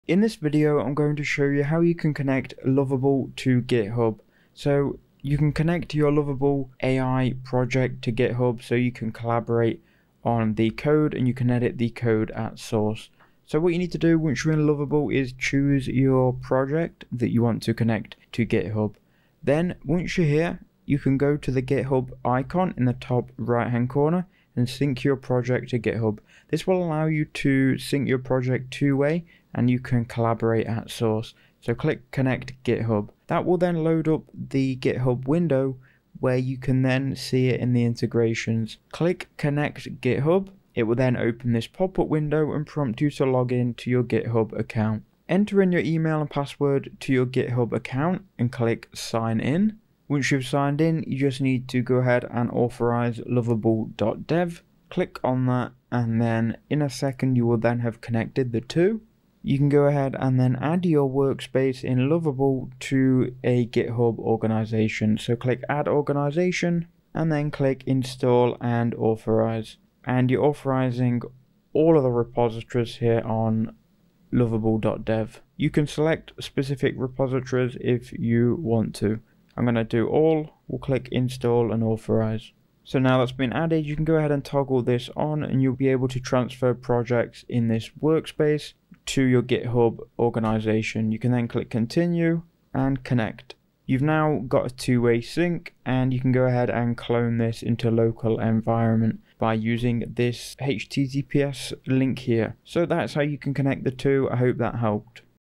How To Connect Lovable To Github (Step By Step)
In this video, I show you how to connect Lovable to Github. Did this video help? Thanks!